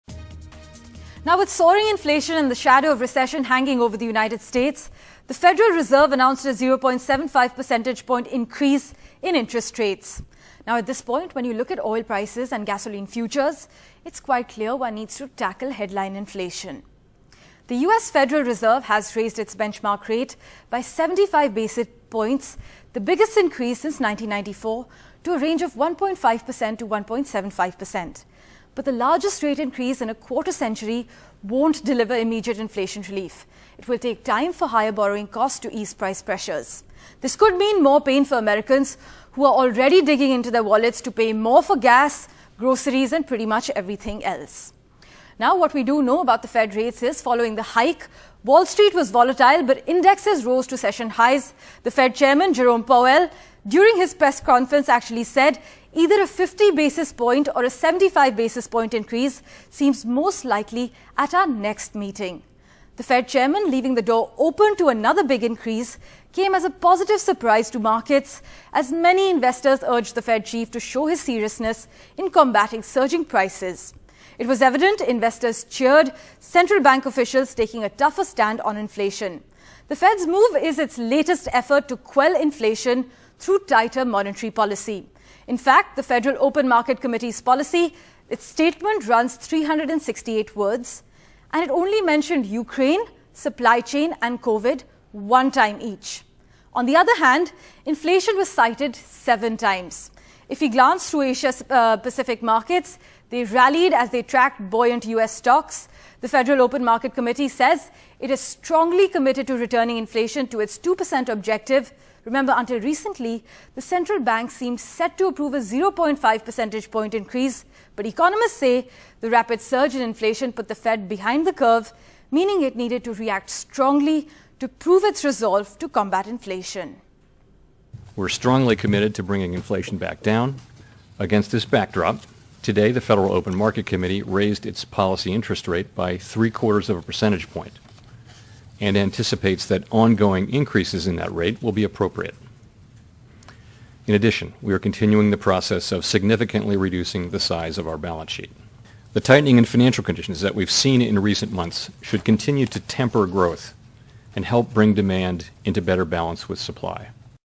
US Federal Reserve Hikes Rates By 75 Basis Points: Key Takeaways
Despite the shadow of a recession hanging over the United States, the Federal Reserve announced a 0.75 percentage-point increase in interest rates, the biggest since 1994, to tackle a red-hot and soaring US inflation, NDTV's Parmeshwar Bawa explains.